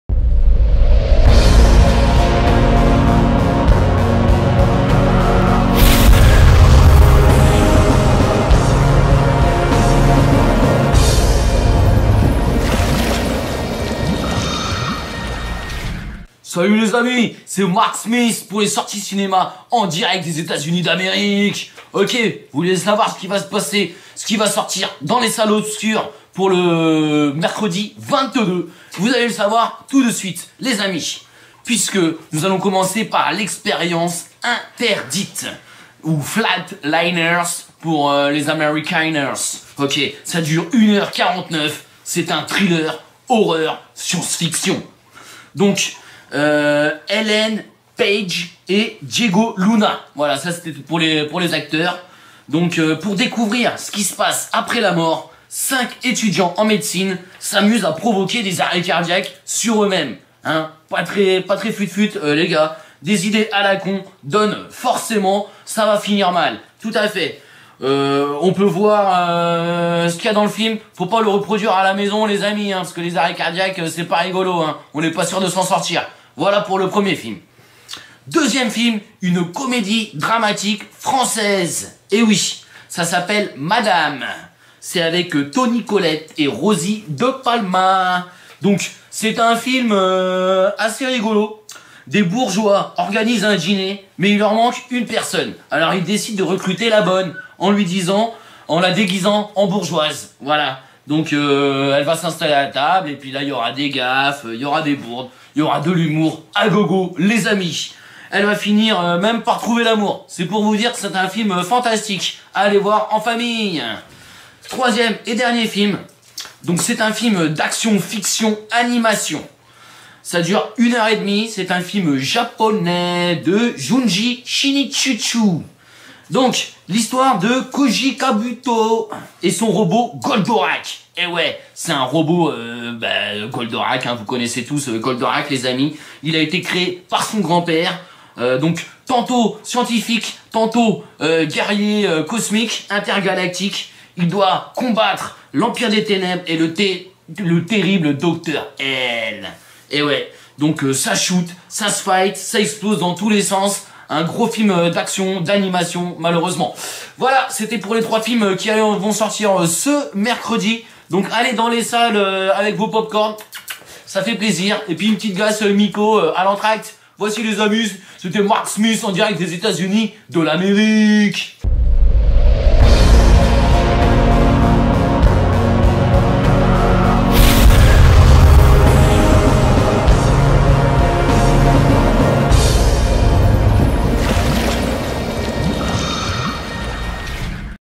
Les Sorties Ciné du 22 Novembre
Découvrez les Sorties CINÉ du 22 Novembre.
Les Sorties Cinéma du 22 Novembre sont sélectionnées par Marc Smith dans l'émission "Manek News"